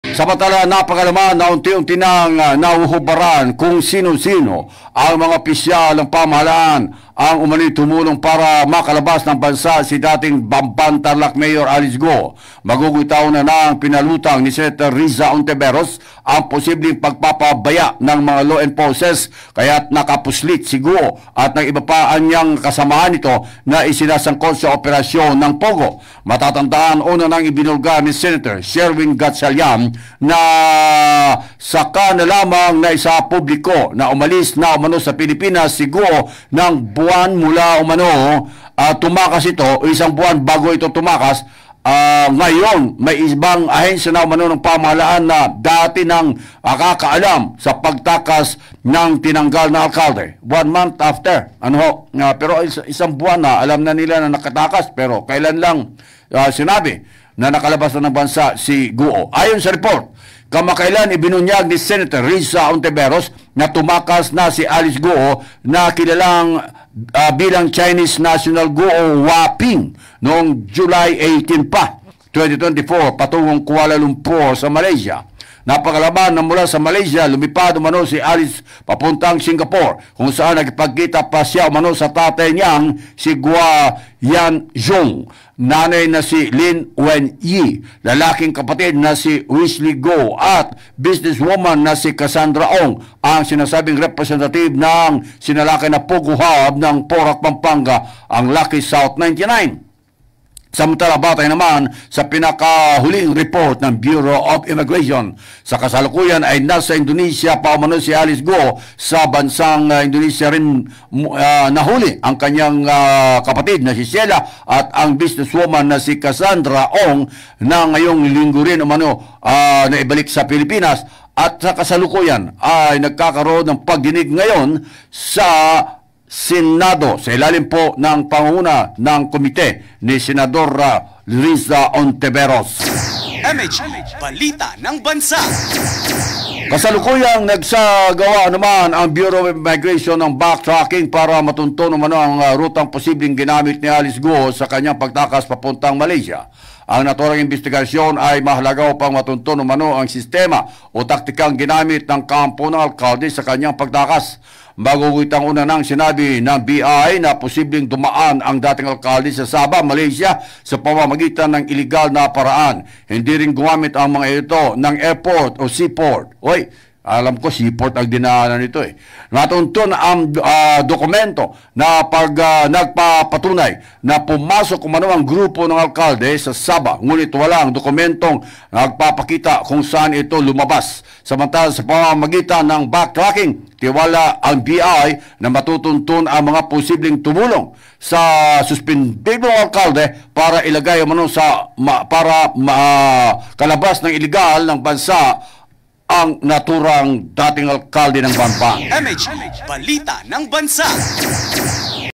News Recap: RUTANG GINAMIT NI ALICE GUO, INAALAM NA NG IMMIGRATION | 08-27-
News Recap: RUTANG GINAMIT NI ALICE GUO, INAALAM NA NG IMMIGRATION; PAGKANSELA SA PASA PORTE NG DATING ALKALDE, MAAARING PAKINABANGAN NITO - PAOCC

Report Date: August 27, 2024 (Tuesday)